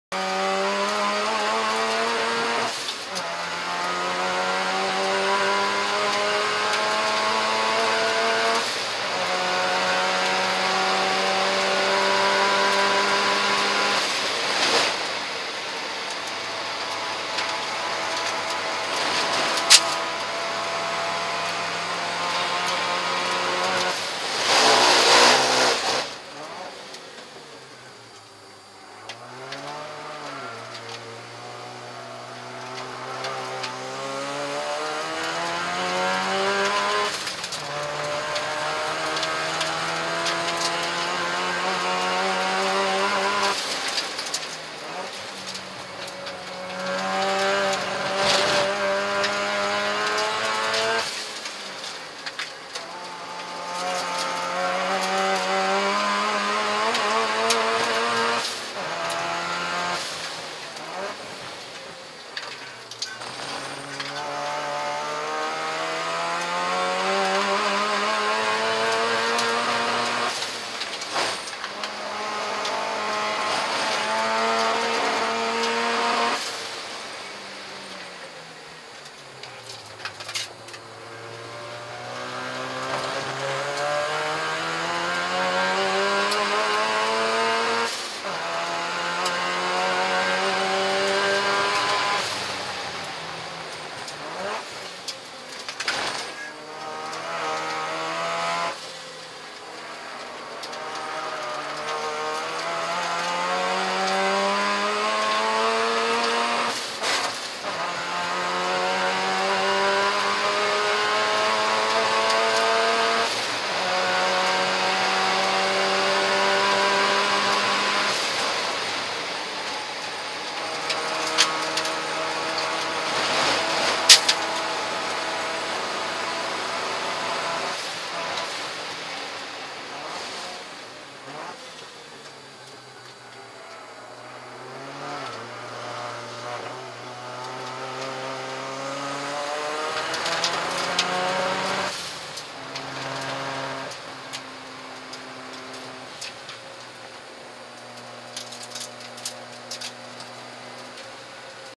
Ric Shaw MX5 Turbo at Eastern Creek
"Yes this is my MX5 and No I won't cut your hair". So says the sticker on the rear window as Ric Shaw punts the little MX5 around Eastern Creek.